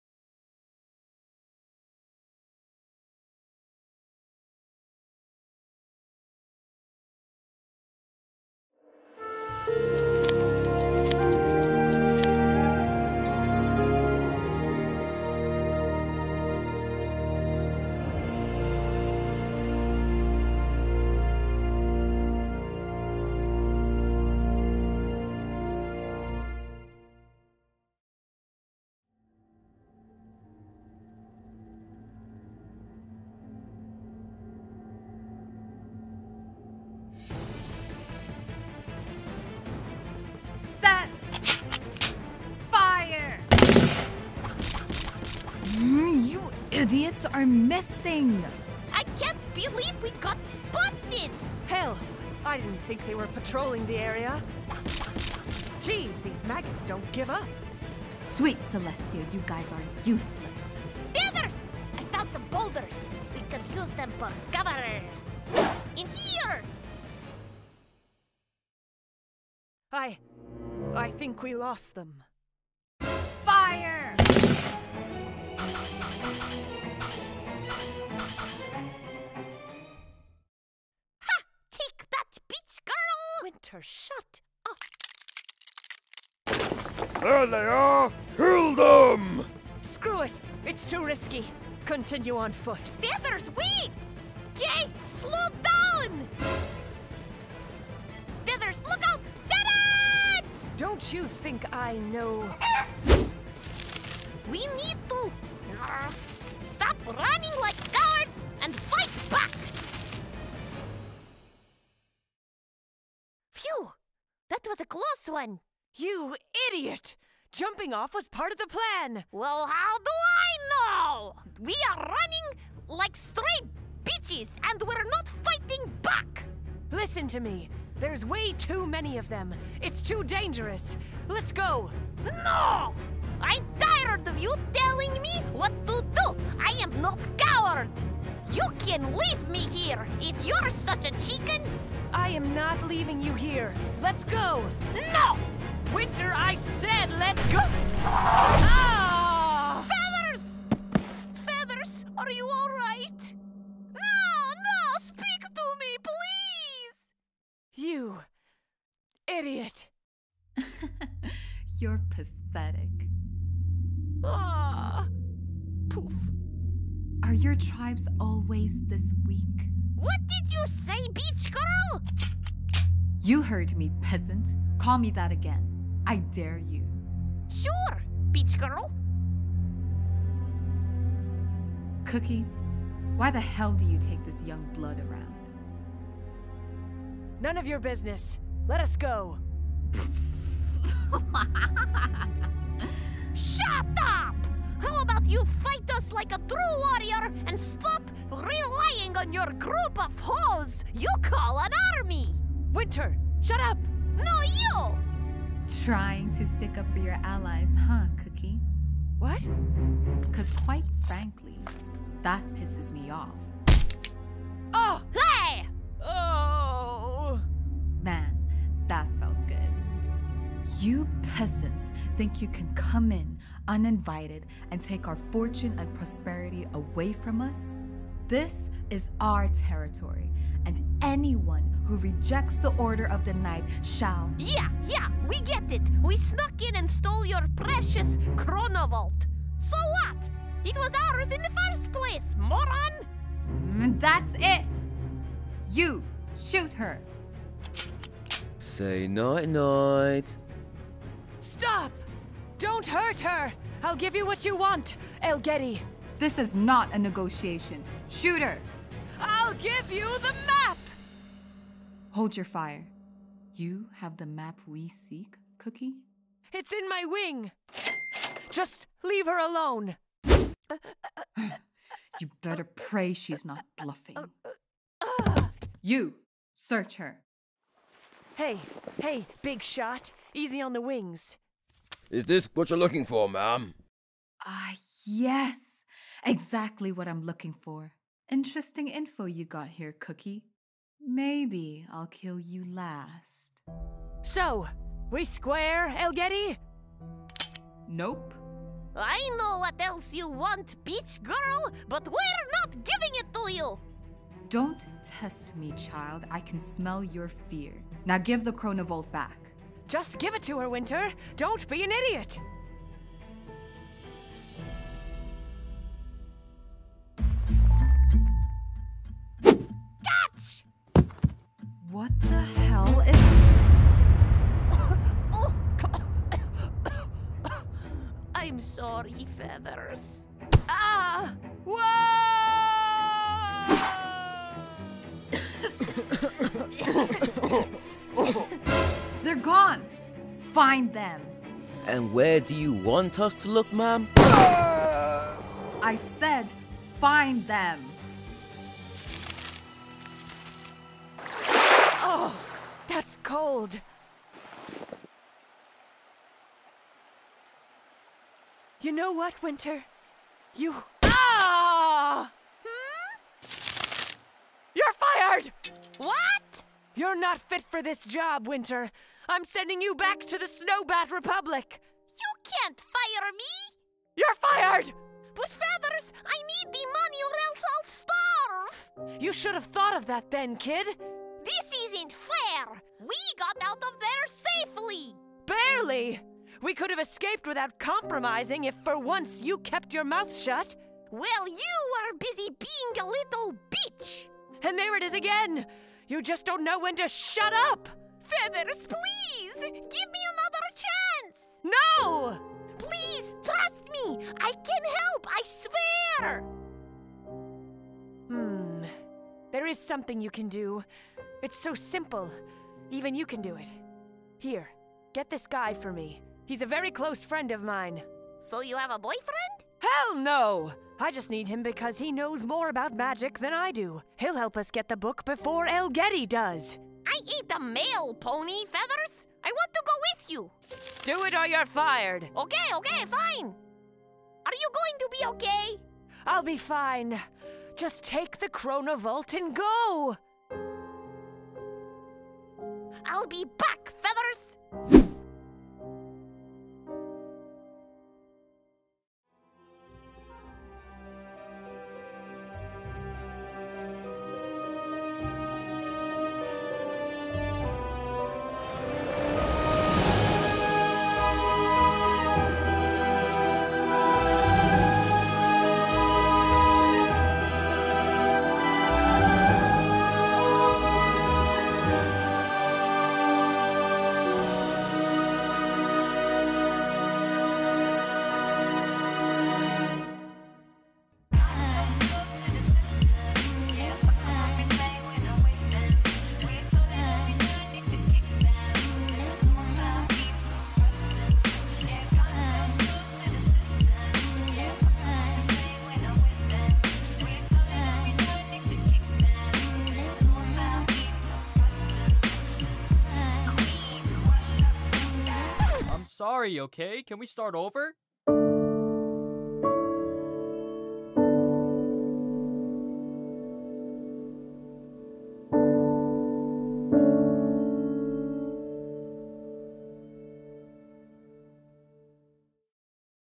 Colt Dudman And The Frozen Scriptures [Chapter 1 DUB] (Comic Dub)
Welcome to the premiere of the first-ever Colt Dudman and the Frozen Scriptures dub! This entire comic is fully voiced will crossfade effects, music, and everything you can ever ask for!

This comic dub was definitely a journey to complete. I expected that though, considering the amount of mlp audio dramas I’ve seen over the years I expect this to take as long if not longer.

This is more of a.. uhh.. mlp comic dub drama lol cause it obviously was meant to be read as an mlp comic book than being watched in a video.

BUT, the greatest thing is I basically just improved upon the existing mlp audio dramas in the fandom and made it better in every way possible! Not to mention, the art style is consistent cause I drew everything XDDDD.

For an mlp comic book reading it sure as heck took a long time to edit like I’m talking 21 hours of editing, that’s not even including the time it took to make the assets in the first place.

But hey, for a pony comic that I’m very passionate about I’ll do anything to make projects like this to happen.

I respect those who make comic dub mlp, considering that it's pretty hard work. If it's more than 10 minutes that is XD

I want to personally thank all the talented voice actors for helping me make this happen. They all did a great job!

VOICE ACTORS

Elgeaty

Winter Thorns & Cookie

Order Of The Night Guards

Chapter 2 is still being drawn but if you like to give it a read before the dub gets released, here’s the link!

This mlp comic was illustrated by me, Snogwritts! I finished this chapter last year hehe. Doing my best to get chapter 2 done. Honestly, the timing of this was so good cause chapters take about 6 months to complete and I'm almost done with Chapter 2 XD.

But anyway, thank you all so much for watching and I hope you enjoyed the show. ^w^